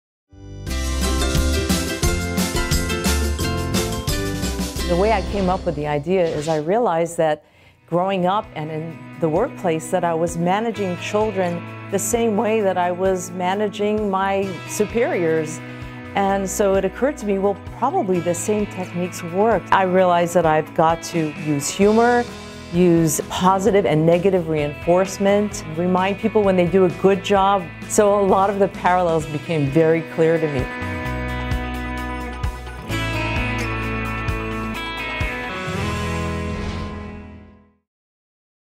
The Lighter Side of Terrible Office Tyrants (TOTs)
Not so surprisingly, the antidote for bad boss and coworker behavior is similar to managing Terrible Two toddlers (without being patronizing, of course!)